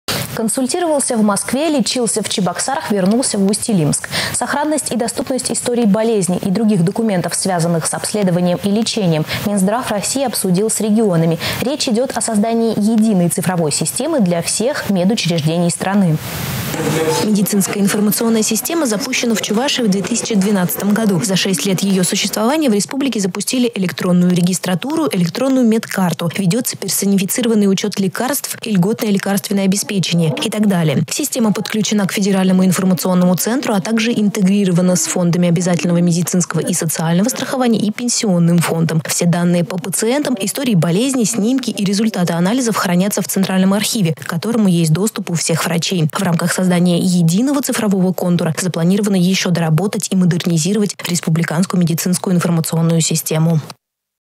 Прошло совещание по созданию единого цифрового контура
Консультировался в Москве, лечился в Чебоксарах, вернулся в Усть-Илимск. Сохранность и доступность историй болезни, других документов, связанных с обследованием и лечением,  Минздрав России обсудил с регионами. Речь идет о создании единой цифровой системы для всех медучреждений страны.